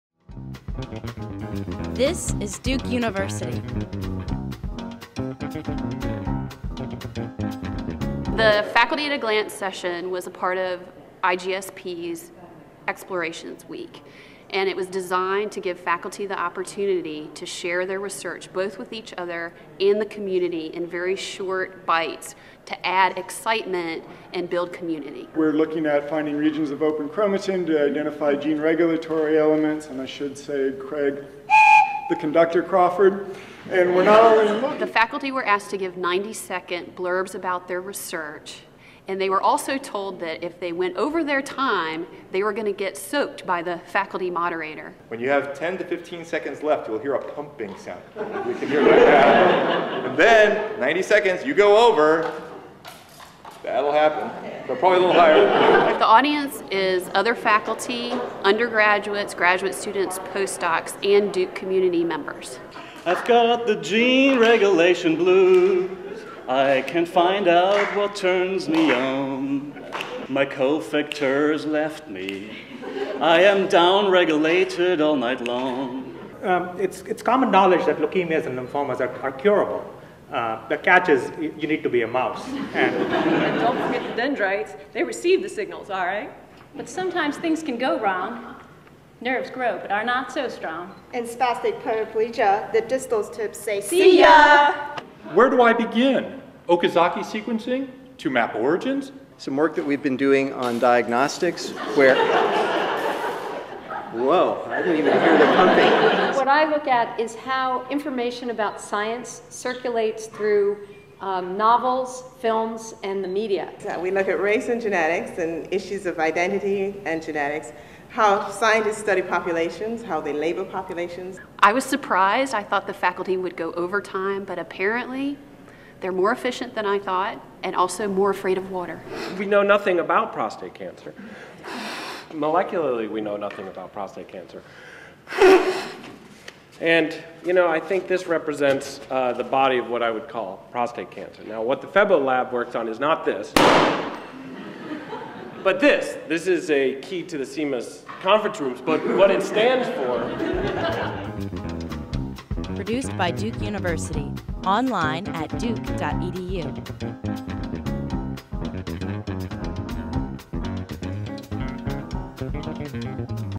Speed-Dating for Science
Faculty in the Duke Institute of Genome Sciences & Policy were asked to describe their work in just 90 seconds, or suffer soggy consequences.